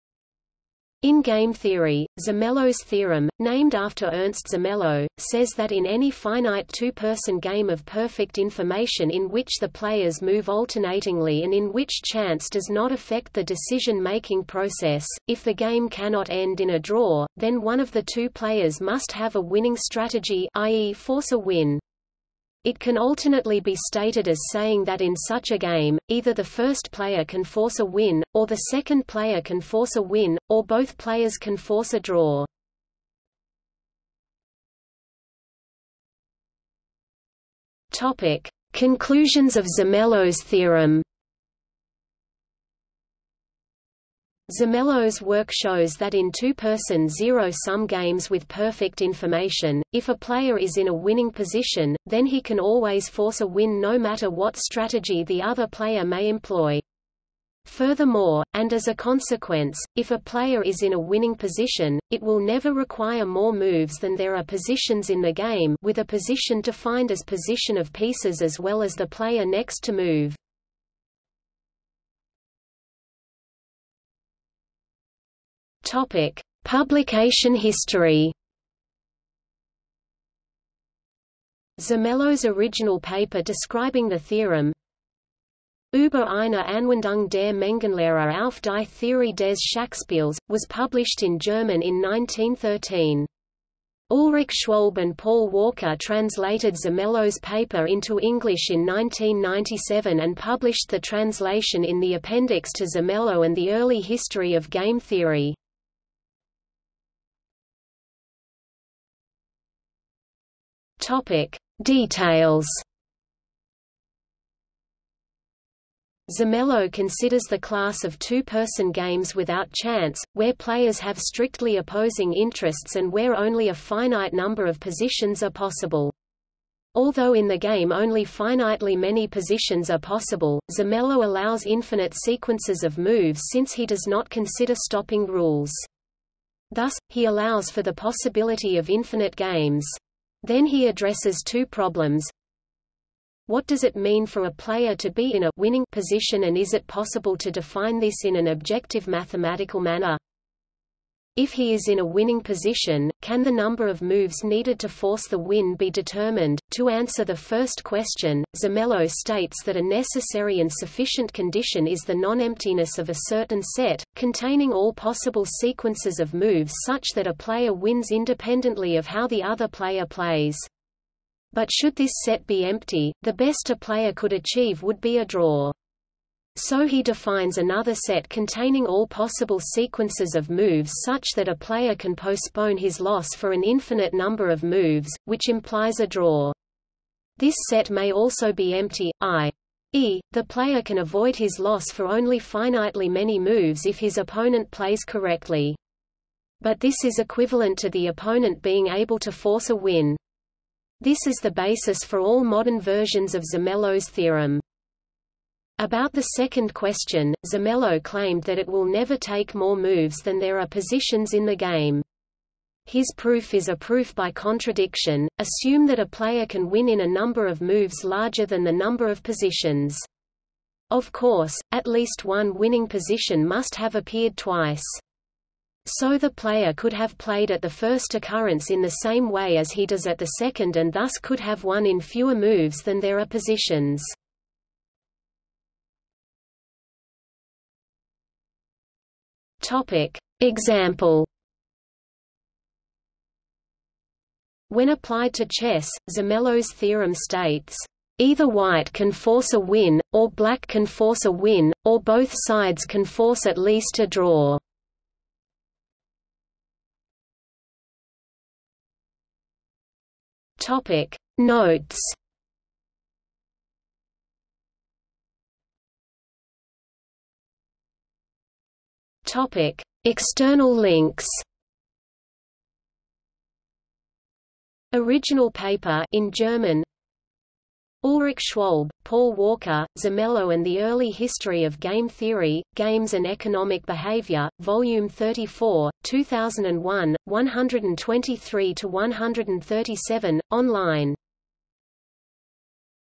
Zermelo's theorem (game theory) | Wikipedia audio article
00:00:34 1 Conclusions of Zermelo's theorem
00:01:12 2 Publication history
00:01:47 3 Details
00:03:56 4 Example
00:04:17 5 Notes
00:04:25 6 External links

- Socrates

SUMMARY
In game theory, Zermelo’s theorem, named after Ernst Zermelo, says that in any finite two-person game of perfect information in which the players move alternatingly and in which chance does not affect the decision making process, if the game cannot end in a draw, then one of the two players must have a winning strategy (i.e. force a win). It can alternately be stated as saying that in such a game, either the first-player can force a win, or the second-player can force a win, or both players can force a draw.